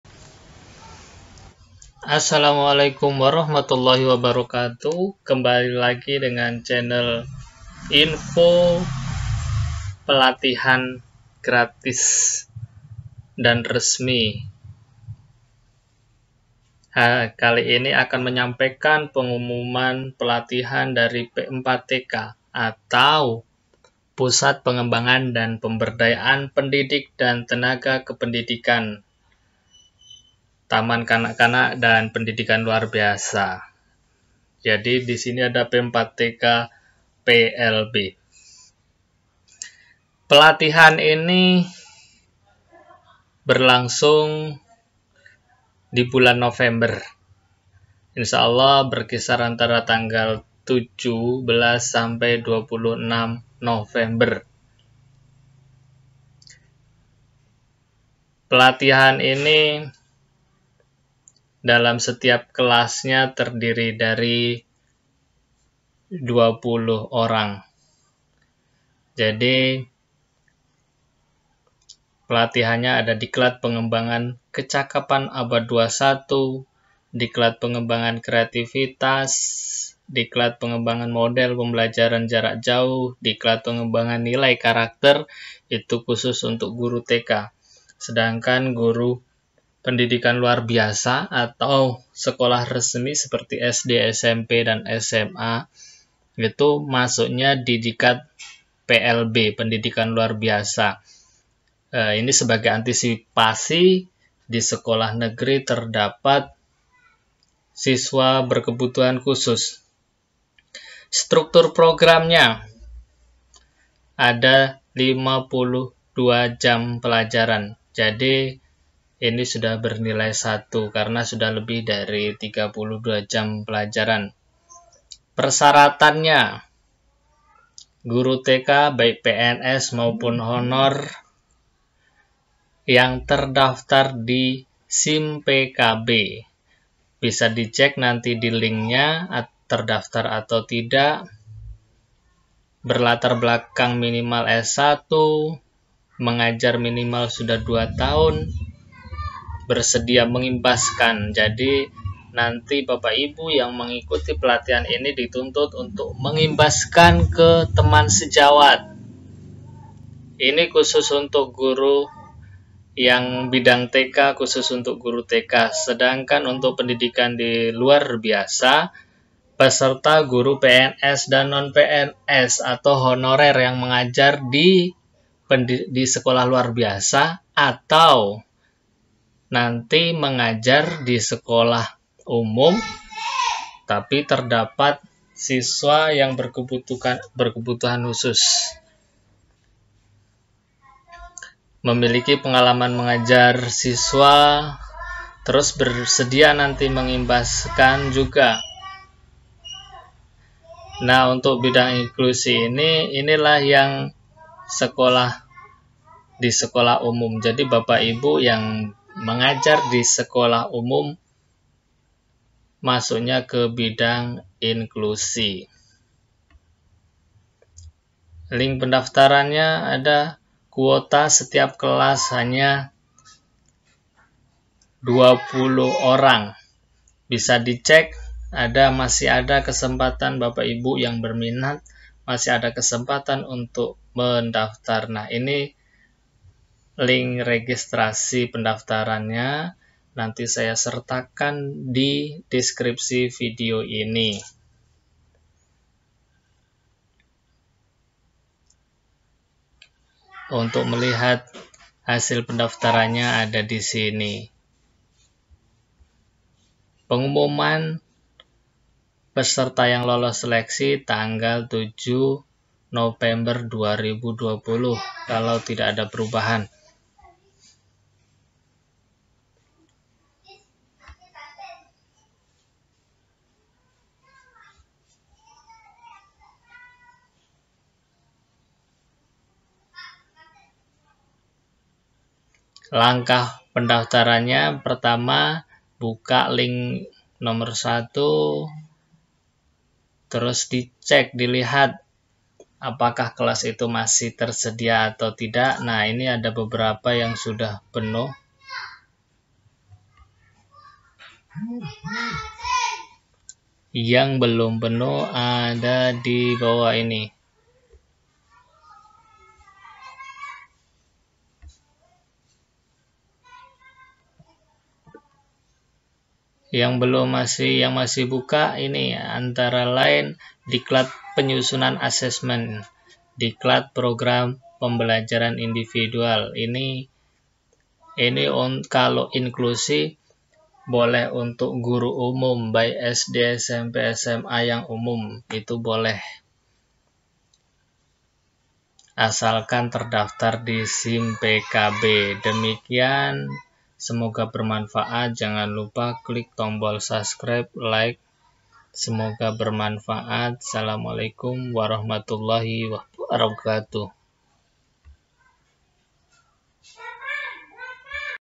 Pelatihan Guru Bersertifikat 52 JP Gratis
Pelatihan Guru Bersertifikat 52 JP  Gratis ini resmi dari P4TK TK SLB Kemdikbud
Info Pelatihan Guru gratis bersertifikat
PERSYARATAN PESERTA
BIDANG TK
• Peserta adalah Guru TK, PNS atau Non PNS atau Honorer yang terdaftar pada SIMPKB [cek sebagai Guru TK;
• Memiliki Pengalaman mengajar minimal 2 tahun di TK;
• Latar Belakang Pendidikan minimal S-1;
• Bersedia Mengimbaskan hasil pelatihan kepada teman sejawat;
• Diijinkan oleh atasan langsung untuk mengikuti kegiatan di PPPPTK TK dan PLB;
• Bersedia mengikuti seluruh rangkaian diklat daring sampai selesai;
• Belum pernah mengikuti Diklat di P4TK TK dan PLB baik Daring maupun Luring di tahun 2020;
• Tidak sedang melaksanakan diklat maupun seleksi PPG dan PPGP;
• Surat Tugas dari Kepala Sekolah atau Yayasan.
• Memiliki Laptop dan Gawai yang terkoneksi dengan Internet

BIDANG PLB
• Peserta adalah Guru PNS dan Non PNS atau Honorer yang mengajar di Sekolah Luar Biasa dan menangani siswa Tuna Netra/Tuna Rungu/ Tuna Daksa/ Tuna Grahita /Autis;
• Terdaftar pada simpkb [cek sebagai Guru SLB;
• Memiliki pengalaman mengajar siswa Tuna Netra / Tuna Rungu / Tuna Daksa / Tuna Grahita / Autis minimal 3 (tiga) tahun;

BIDANG INKLUSI
• Peserta adalah Guru PNS dan Non PNS atau honorer yang mengajar di Sekolah Umum;
• Terdaftar pada SIMPKB [cek sebagai Guru;

PENDAFTARAN
Pendaftaran akan dibuka mulai tanggal 3 November 2020 pukul 08.00 WIB (pagi), sampai dengan 5 November pukul 16.00 WIB (sore) atau sampai kuota terpenuhi maka pendaftaran otomatis ditutup.

2. Lihat Kolom VALID dan STATUS, Pendaftar Valid adalah pendaftar memenuhi persyaratan (sebelum diverifikasi)
3. Jika Kuota Belum terpenuhi (STATUS = OPEN), silahkan isi form pendaftran pada link s.id/reg-pst-tkplb-20
4. Jika Kuota sudah terpenuhi (STATUS = CLOSE), silahkan pilih kegiatan lain
5. Lihat hasil Pendaftaran Anda di pada sheet LOG
(Merah atau ungu menandakan pendaftaran anda tidak valid)

PENGUMUMAN HASIL SELEKSI
Peserta yang lolos seleksi akan dimuat maksimum pada tanggal 7 Nopember 2020 pada pengumuman ini juga.